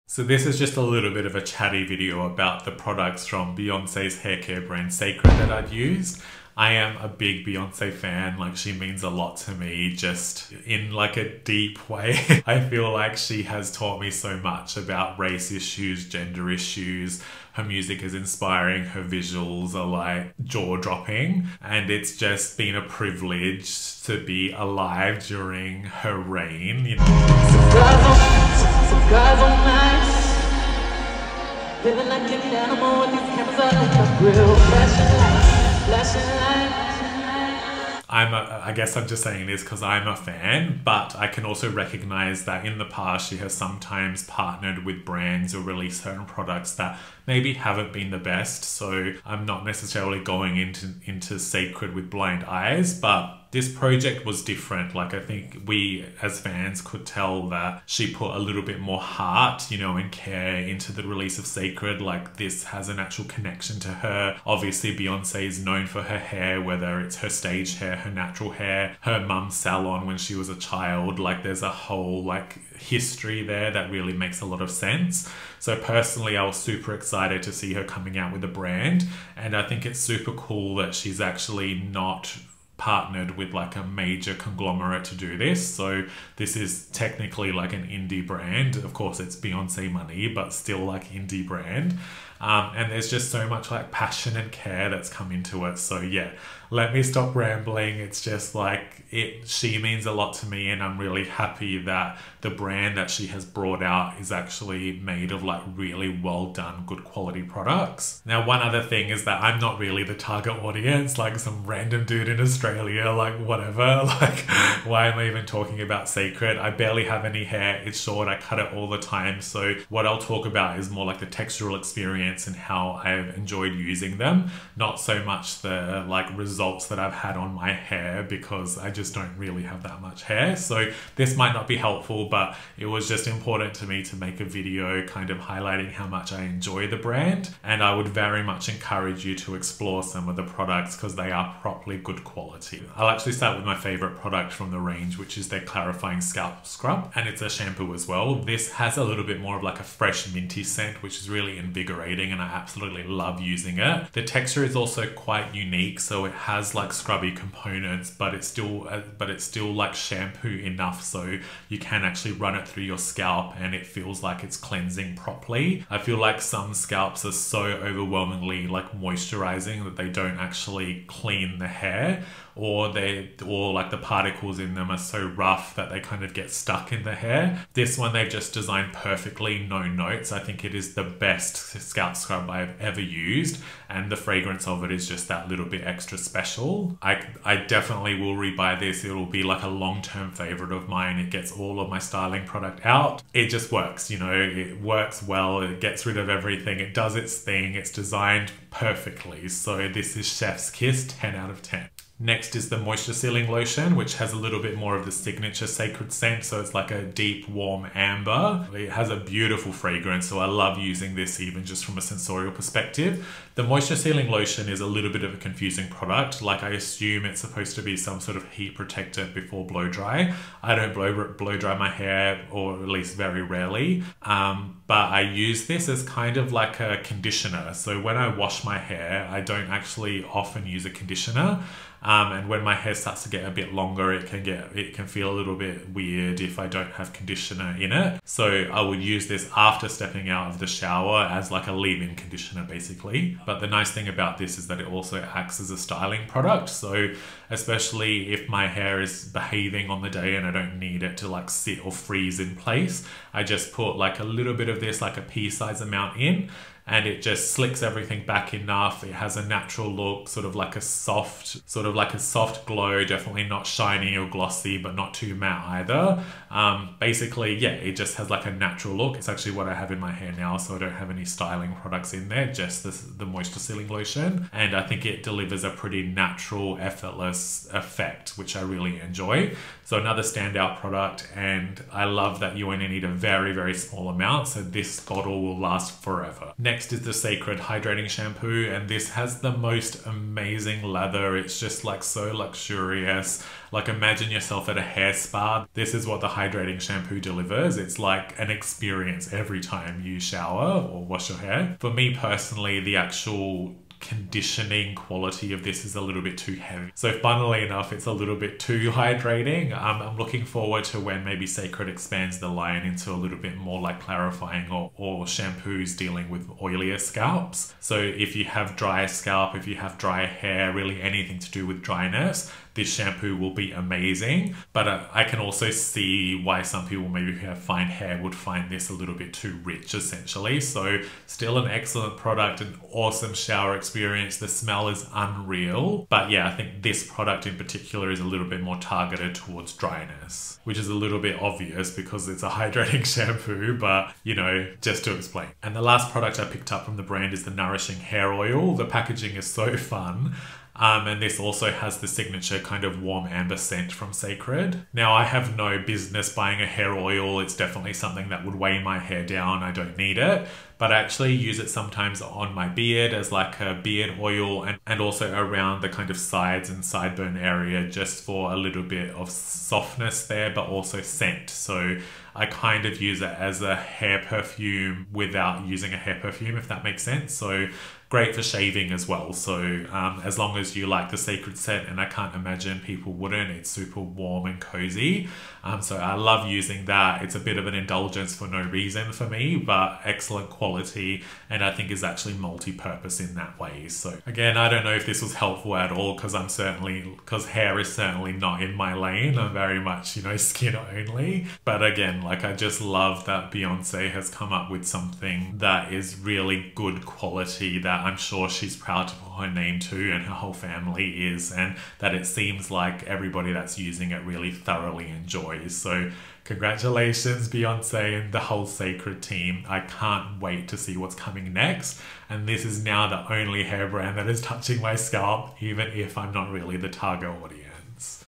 Cécred by Beyoncé is actually great 🐝 Review
Cécred by Beyoncé is an outstanding haircare brand, and I can't wait to see what they have coming up.

This video is not sponsored and all products were purchased myself.

Products featured:
Clarifying Shampoo & Scalp Scrub
Moisture Sealing Lotion
Hydrating Shampoo
Nourishing Hair Oil

Timestamps:
00:00 Introduction
02:22 Clarifying Shampoo & Scalp Scrub
03:22 Moisture Sealing Lotion
04:47 Hydrating Shampoo
05:45 Nourishing Hair Oil
06:34 Outro